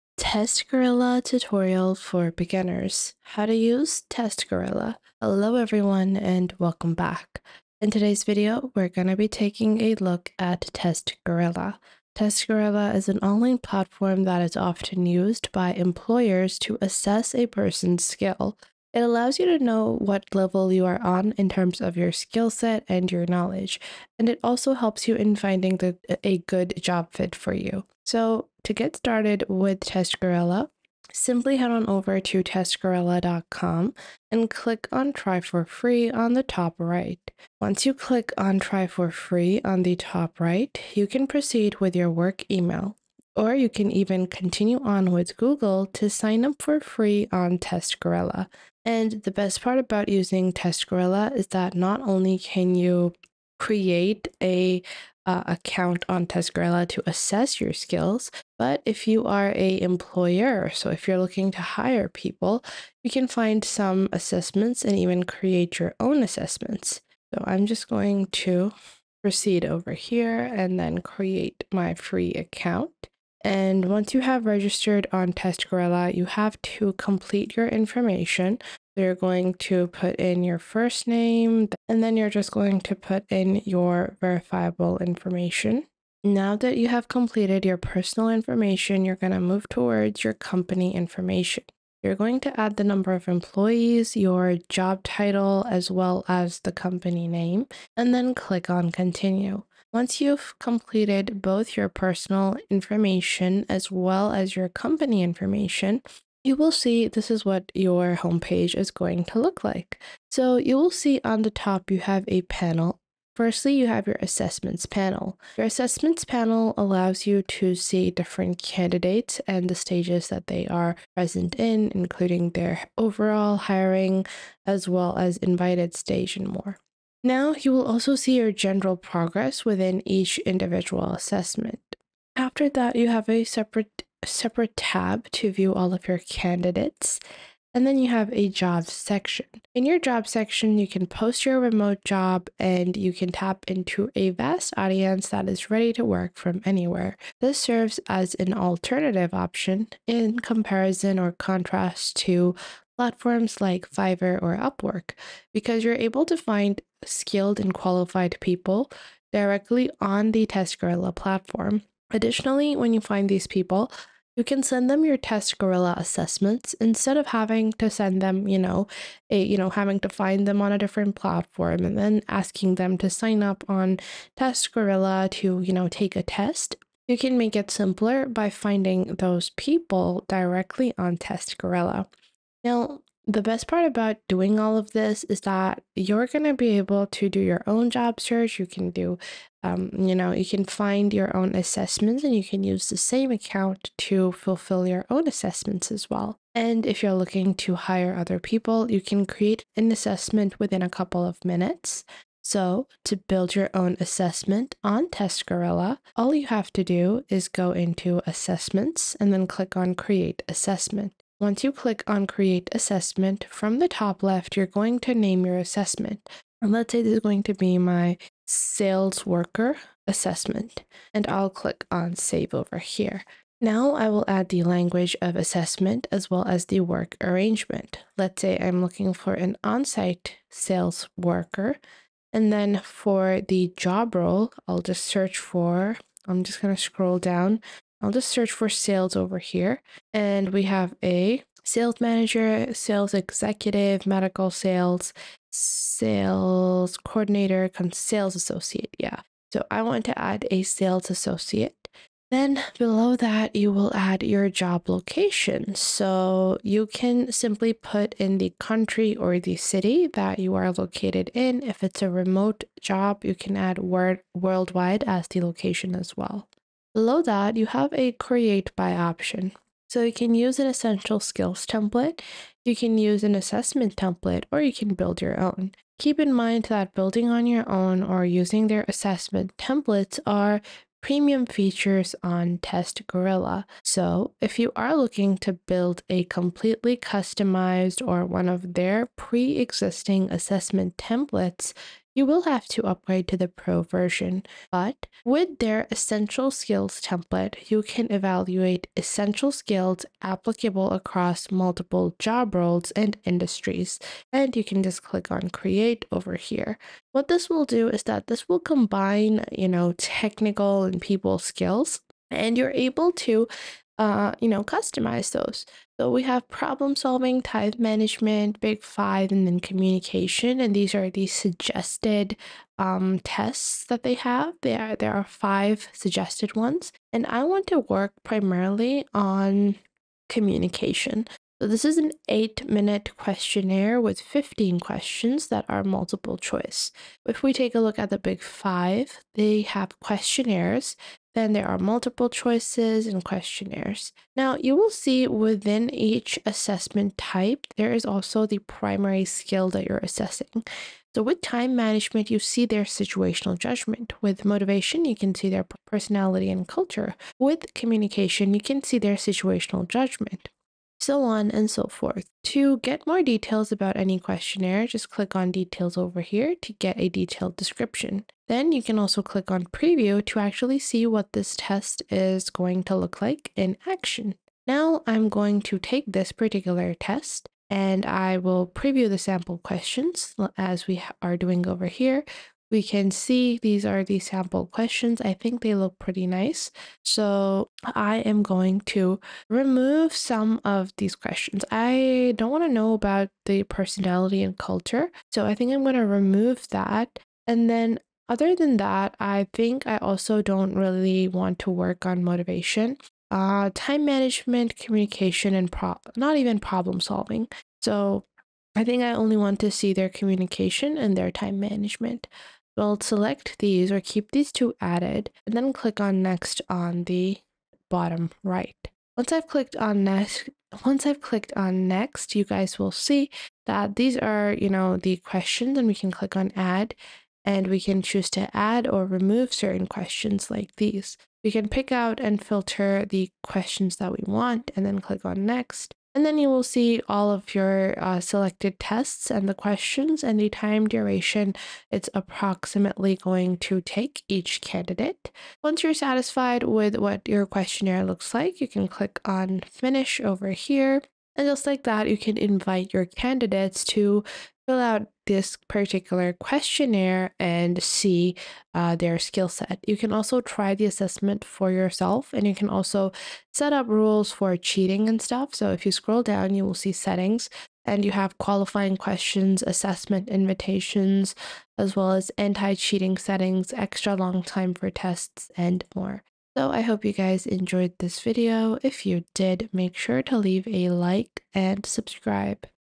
TestGorilla Tutorial For Beginners | How To Use TestGorilla
The "TestGorilla Tutorial for Beginners" serves as an essential guide for individuals looking to leverage the TestGorilla platform effectively. This comprehensive tutorial walks users through the process of creating, administering, and analyzing pre-employment tests tailored to specific roles. Beginners will learn how to navigate the user-friendly interface, customize assessments, and interpret results to make informed hiring decisions.

Additionally, the tutorial highlights best practices for utilizing TestGorilla's extensive library of tests, ensuring that organizations can enhance their recruitment strategies. By following this guide, users will gain the confidence and skills necessary to optimize their hiring processes through data-driven insights.